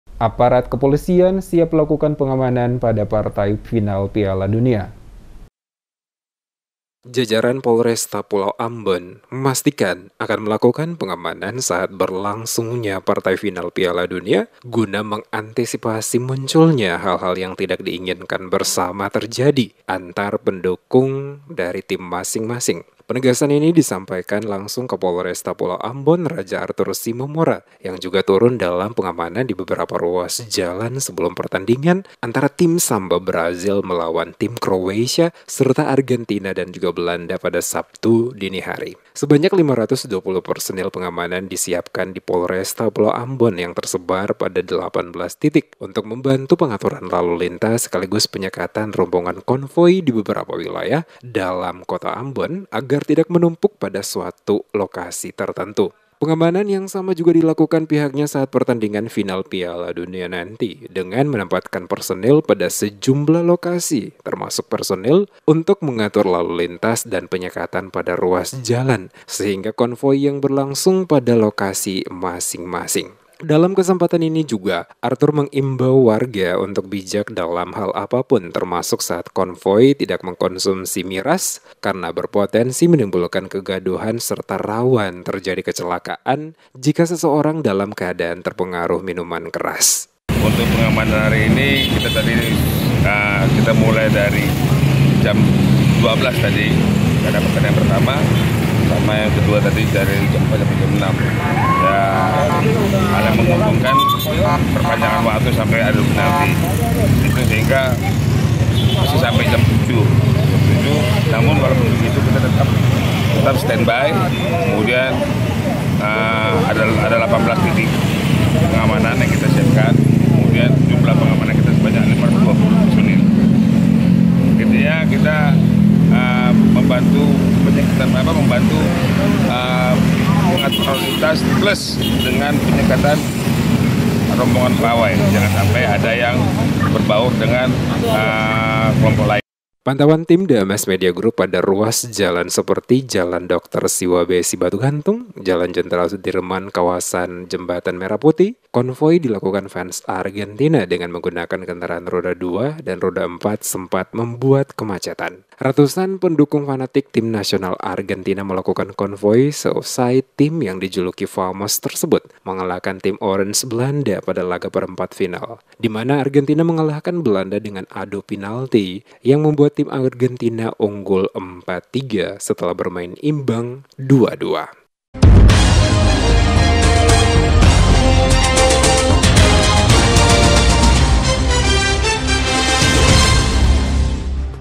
Polisi Siap Lakukan Pengamanan Partai Final Piala Dunia
Berita Ambon - Jajaran Polresta pulau Ambon memastikan akan melakukan pengamanan saat berlangsungnya partai final piala dunia guna mengantisipasi munculnya hal-hal yang tidak diinginkan bersama terjadi antar para pendukung tim masing-masing.

Penegasan ini dismapikan langsung Kapolresta pulau Ambon Raja Arthur  Simamora yang juga turun dalam pengamanan dibeberapa ruas jalan sebelum pertandingan antara Tim Samba Brasil melawan Tim Croasia serta Argentina dan Belanda pada Sabtu dini hari.